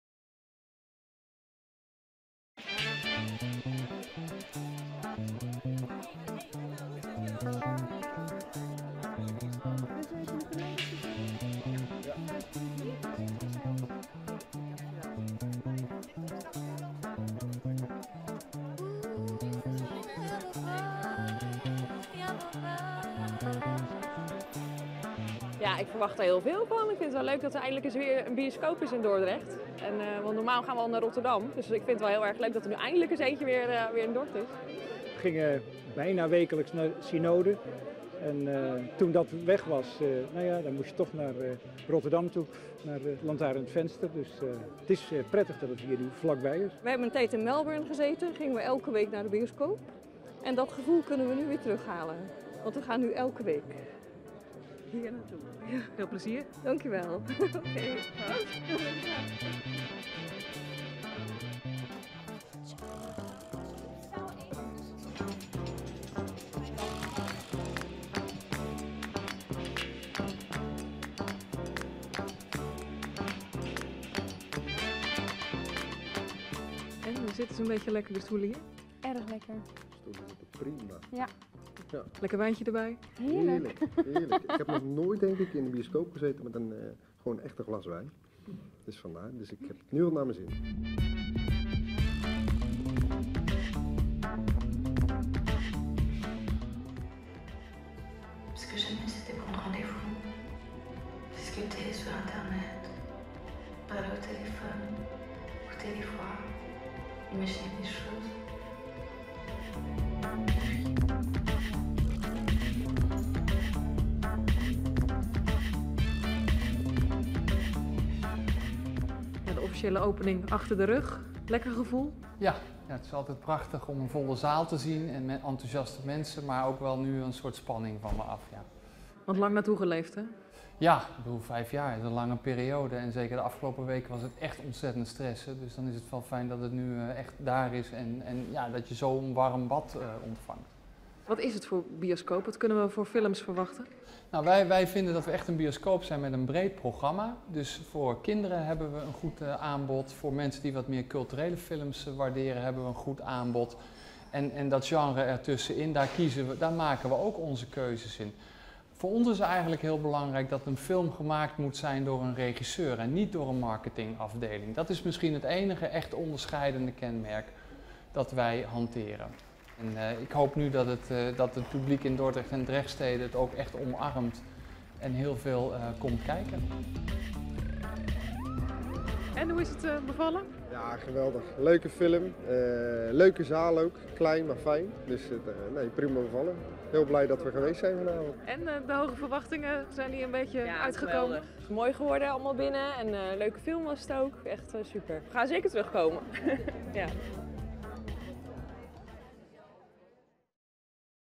Dordtenaren blij met terugkeer bioscoop
Dordt heeft weer een bioscoop. Zeven jaar na de sluiting van Euro Cinema is er nu The Movies. Het grote publiek kon woensdag voor het eerst weer een filmpje pakken.

   Lees meer: Dordtenaren blij met terugkeer bioscoop